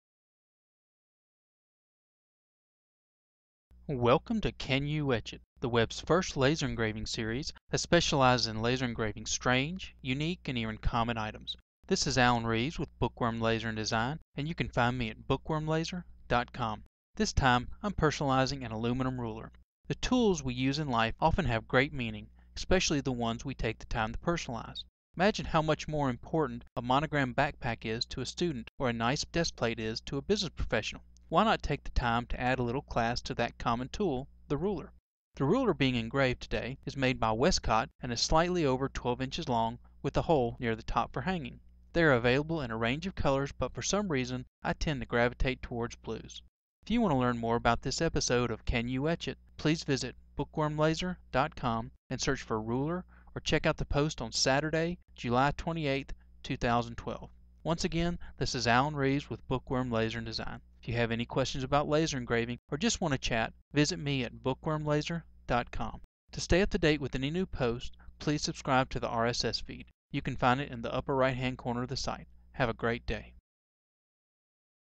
Can You Etch It - Laser Engraved Anodized Aluminum Ruler
Welcome to another episode of Can You Etch It by Book Worm Laser & Design. This is Alan Reeves and today I will customize that great old tool, the ruler.